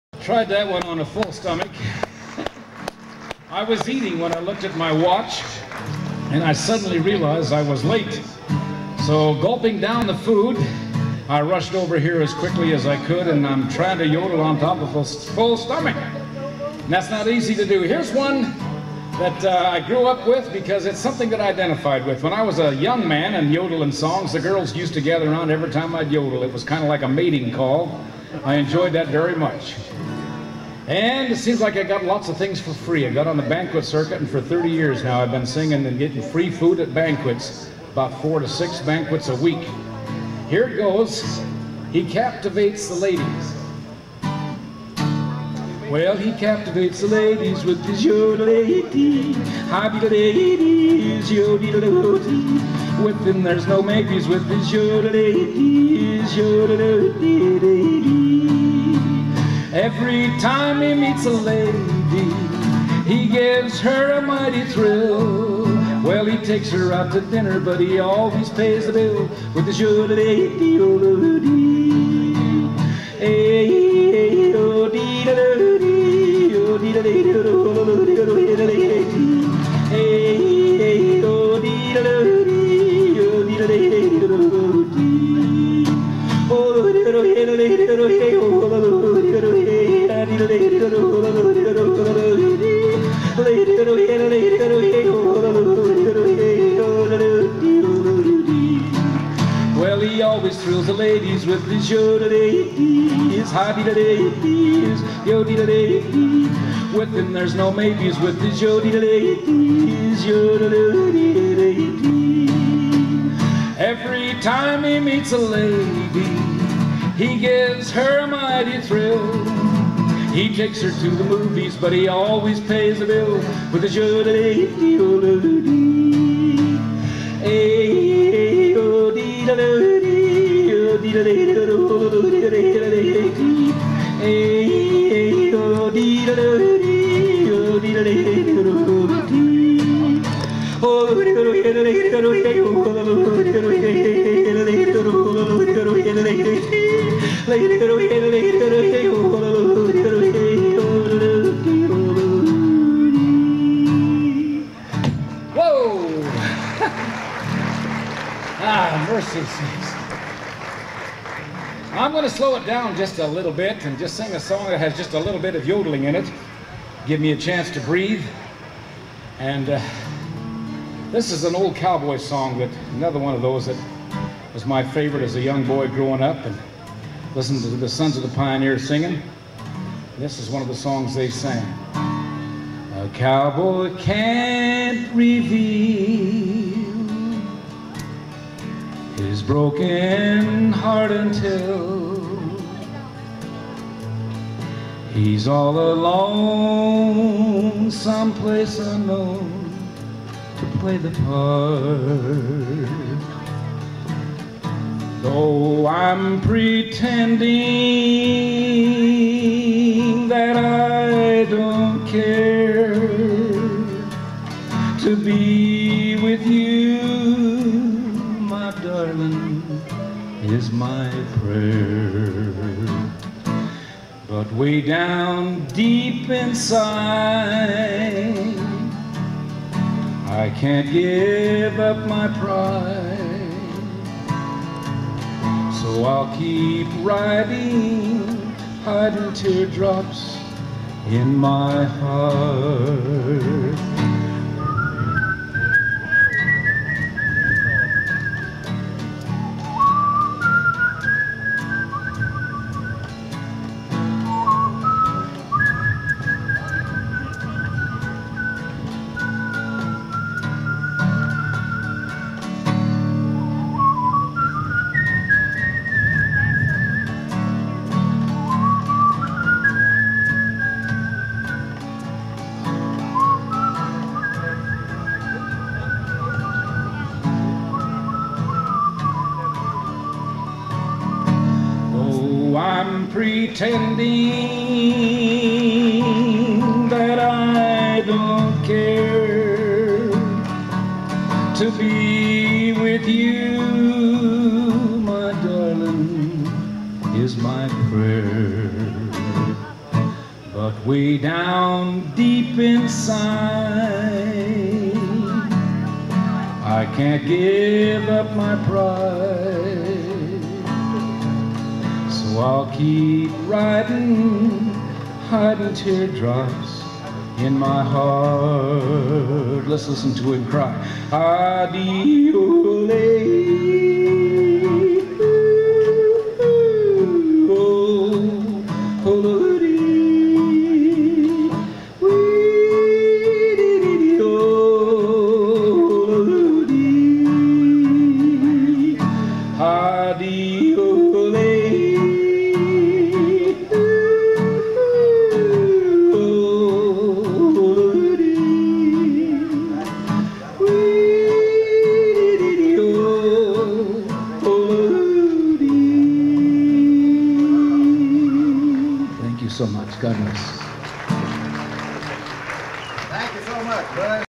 Buzz Goertzen The Idaho Yodeler
Buzz is another one of those high-power Yodelers that draws a crowd where-ever he performs. Juggling his time between his farm work and music, Buzz has released over 20 albums during his illustrious career that spans several decades.

Sonny Rodgers Yodelers Paradise Show at the August 2000 Avoca Old Time Country Music Festival. This 3-hour show was one of Avoca's most popular attractions featuring Yodelers from around the world. In spite of week-long temperatures relentlessly hovering between 97 and 100 degrees, the performers gave it their all and the fans endured.

These excerpted clips represent some of the best performances. There are some audio glitches, of which I wasn't aware until I got home and watched the playbacks. Out of 10 film cassettes I shot, two of them suffered from this defect.

This would also be Sonny Rodgers last Avoca attendance. His cancer reappeared and the Good Lord called him home in July 2001.